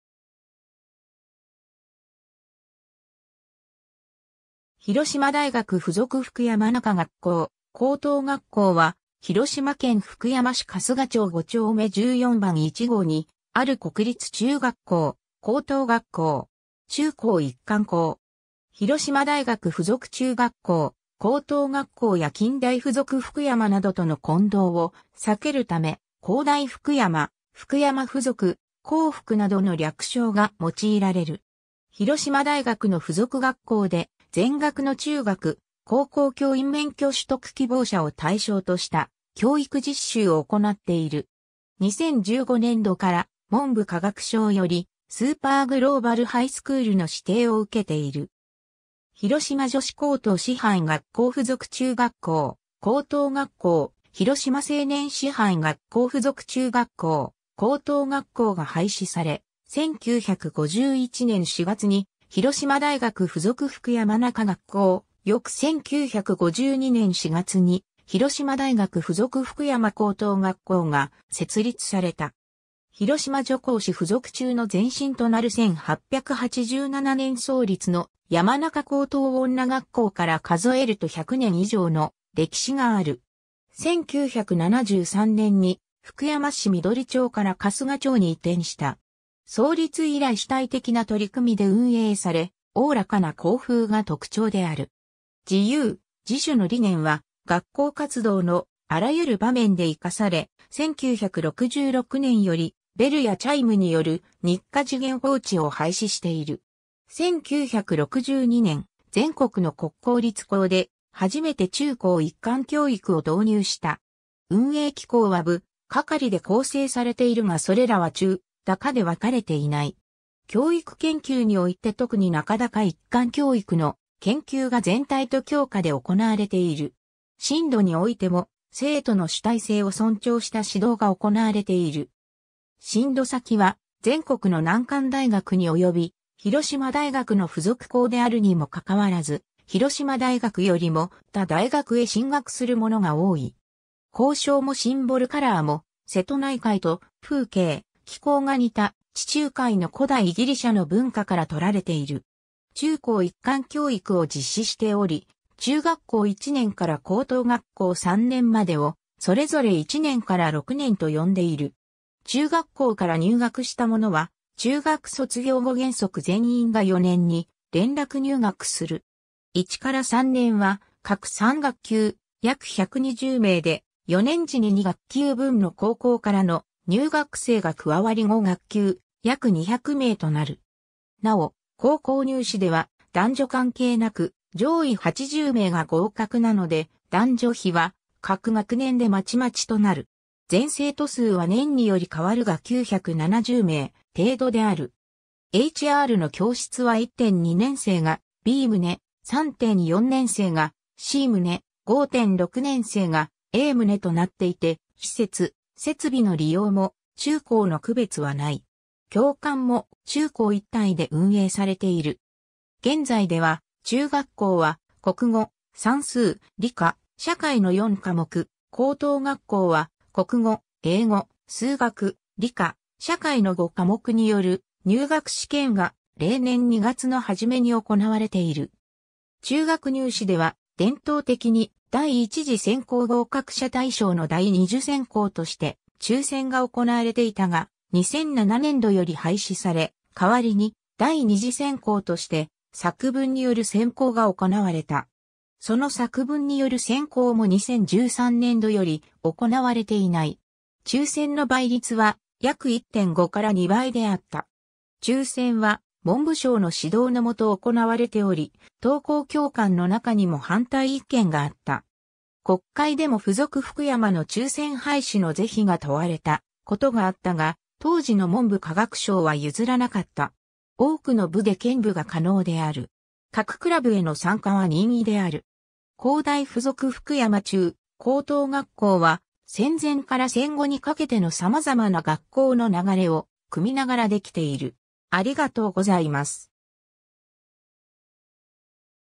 広島大学附属福山中学校・高等学校
広島大学附属福山中学校・高等学校（ひろしまだいがくふぞくふくやまちゅうがっこう・こうとうがっこう, Fukuyama Junior and Senior High School Attached to Hiroshima University）は、広島県福山市春日町五丁目14番1号にある国立中学校・高等学校。
中高一貫校。
 広島大学附属中学校・高等学校（「本校」と呼ばれる）や近大附属福山（「近大」と呼ばれる）などとの混同を避けるため「広大福山」「福山附属」「広福（ひろふく）」などの略称が用いられる。
 広島大学の附属学校で、全学の中学・高校教員免許取得希望者を対象とした教育実習を行っている。
 2015年度から文部科学省よりスーパーグローバルハイスクール (SGH) の指定を受けている。
 広島女子高等師範学校附属中学校・高等学校、広島青年師範学校附属中学校・高等学校が廃止され、1951年（昭和26年）4月に広島大学附属福山中学校、翌1952年（昭和27年）4月に広島大学附属福山高等学校が設立された。
広島女高師附属中の前身となる1887年（明治20年）創立の山中高等女学校から数えると100年以上の歴史がある。
1973年に福山市緑町から春日町に移転した。
 創立以来主体的な取り組みで運営され、大らかな校風が特徴である。
自由・自主の理念は学校活動のあらゆる場面で生かされ、1966年（昭和41年）よりベルやチャイムによる日課時限報知を廃止している。
 1962年（昭和37年）、全国の国公立校で初めて中高一貫教育を導入した。
運営機構は部・ 係で構成されているがそれらは中・高で分かれていない。
教育研究において特に中高一貫教育の研究が全体と教科で行われている。
 進路においても、生徒の主体性を尊重した指導が行われている。
進路先は全国の難関大学に及び、広島大学の附属校であるにもかかわらず広島大学よりも他大学へ進学する者が多い。
 校章もシンボルカラーも、瀬戸内海と風景、気候が似た地中海の古代ギリシャの文化からとられている。
 中高一貫教育を実施しており、中学校1年から高等学校3年までをそれぞれ1年から6年と呼んでいる。
中学校から入学した者は中学卒業後原則全員が4年に連絡入学する。
1～3年は各3学級、約120名（男子60名、女子60名）で、4年次に2学級分の高校からの入学生が加わり5学級、約200名となる。
 なお、高校入試では男女関係なく上位80名が合格なので男女比は各学年でまちまちとなる。
全生徒数は年により変わるが970名程度である。
 HRの教室は1・2年生がB棟、3・4年生がC棟、5・6年生がA棟となっていて、施設・設備の利用も中高の区別はない。
 教官も中高一体で運営されている。
 現在では、中学校は国語、算数、理科、社会の4科目、高等学校は国語、英語、数学、理科、社会の5科目による入学試験が例年2月の初めに行われている。
 中学入試では伝統的に第1次選考（学科試験）合格者対象の第2次選考として抽選（くじ引き）が行われていたが、2007年度より廃止され代わりに第2次選考として作文による選考が行われた。
その作文による選考も2013年度より行われていない。
 抽選の倍率は約1.5 - 2倍であった。
抽選は文部省（現：文部科学省）の指導の下行われており、当校教官の中にも反対意見があった。
国会でも附属福山の抽選廃止の是非が問われたことがあったが、当時の文部科学省は譲らなかった。
 多くの部で兼部が可能である。
各クラブへの参加は任意である。
 広大附属福山中・高等学校は、戦前から戦後にかけての様々な学校の流れを汲みながら出来ている。